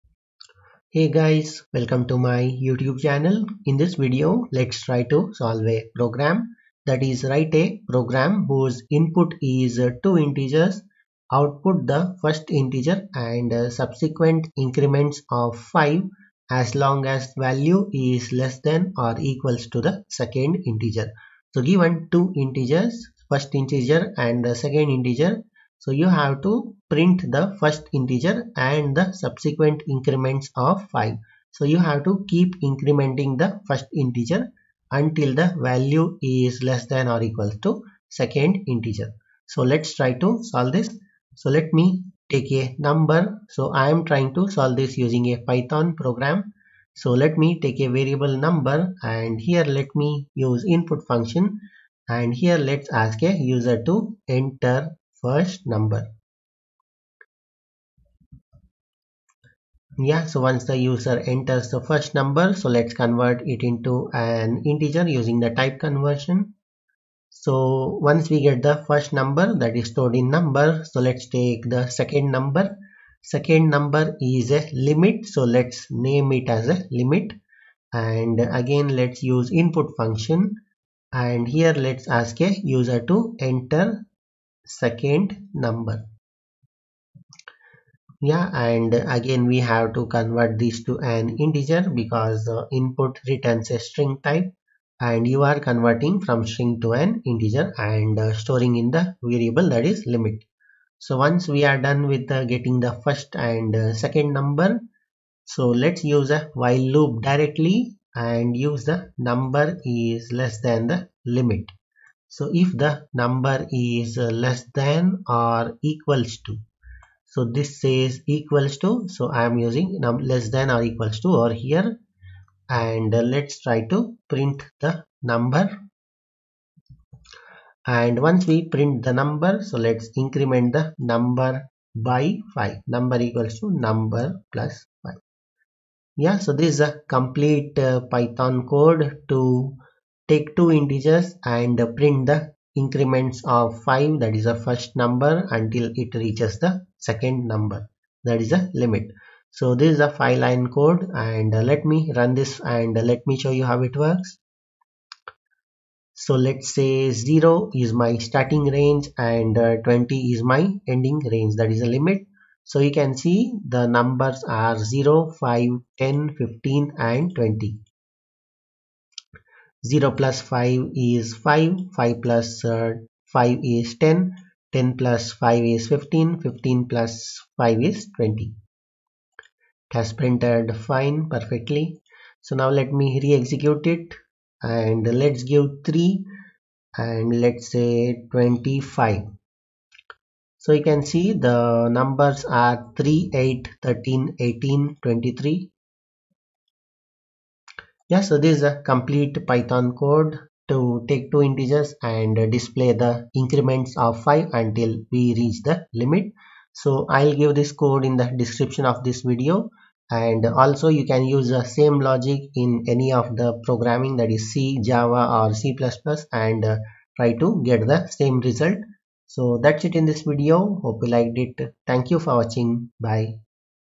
Write a Program Whose Input is Two Integers. Output The First Integer And Subsequent Increments of 5
In this video you will learn about how to Write a Program Whose Input is Two Integers. Output The First Integer And Subsequent Increments of 5 as Long as The Value is Less Than or Equal to The Second Integer

Program Statement
Write a Program Whose Input is Two Integers. Output The First Integer And Subsequent Increments of 5 as Long as The Value is Less Than or Equal to The Second Integer

Code
number = int(input("Enter first number : "))
limit = int(input("Enter second number : "))

while number replace_this_with_less_than_symbol= limit:
    print(number)
    number = number + 5